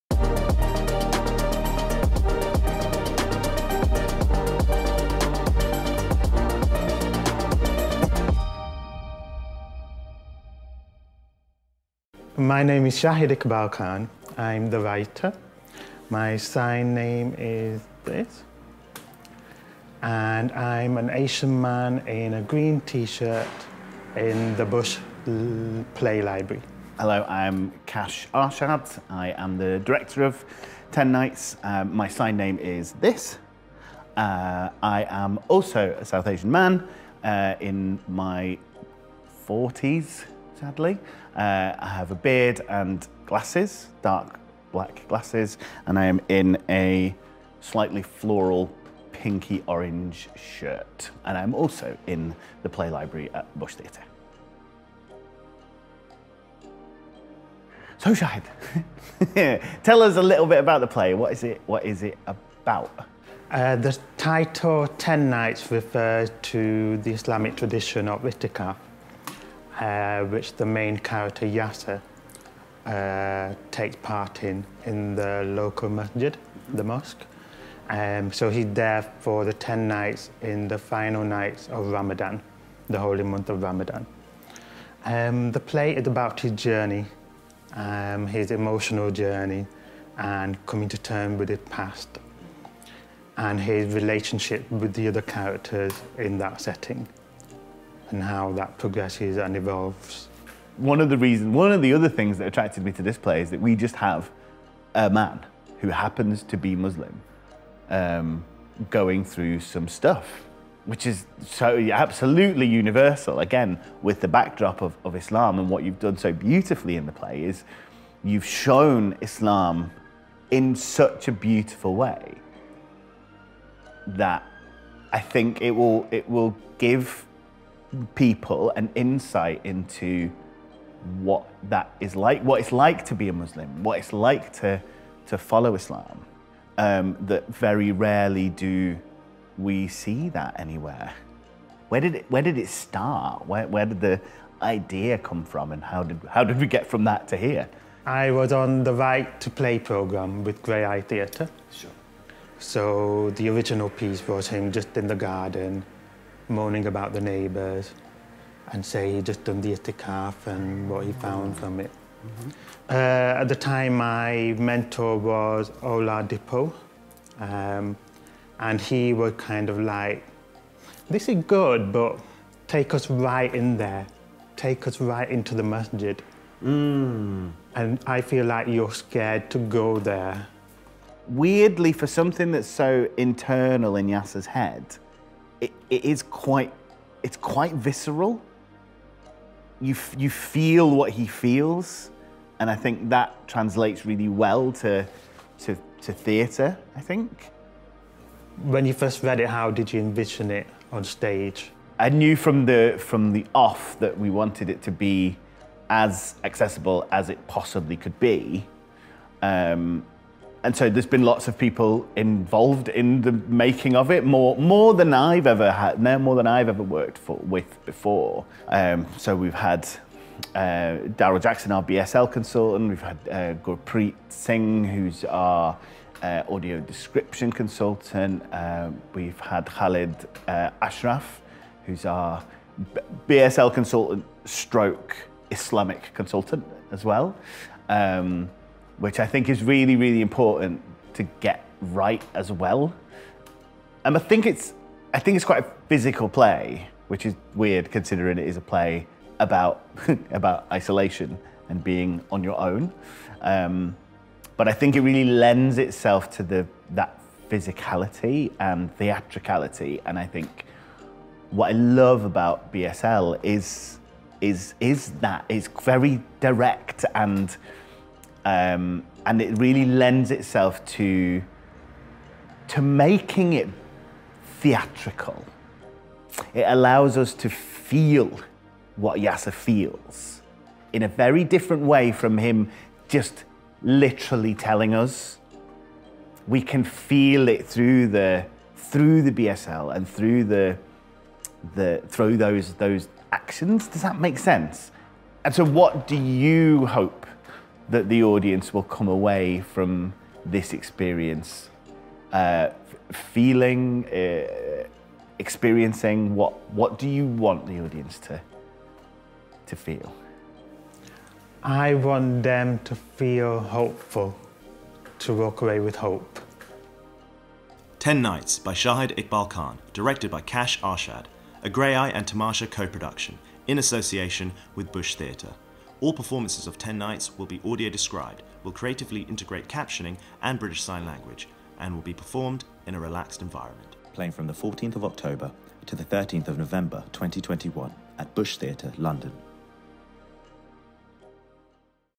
Shahid Iqbal Khan x Kash Arshad | In Conversation
The writer and director of 10 Nights sit down at the Bush Theatre's Script Library to have a chat about the making of 10 Nights, the themes and inspirations for the characters, and what it's like to work with integrated creative access.

10 NIGHTS
by Shahid Iqbal Khan
A Graeae and Tamasha co-production, in association with Bush Theatre
Playing at the Bush Theatre until 14 Nov. Tickets from £10.

One man’s spiritual journey is about to begin… after he’s sent this tweet.

When Yasser decides to take part in itikaf, sleeping and fasting in the mosque for the last ten nights of Ramadan, he soon regrets his decision. But as he navigates smug worshippers, shared bathrooms, and recurring thoughts of chunky chips, Yasser’s isolation forces him to confront a side of himself he’s been trying to keep hidden.

A moving and funny new play by Shahid Iqbal Khan and directed by Kash Arshad, 10 Nights is the story of one man’s journey of self-discovery and facing the consequences of your actions.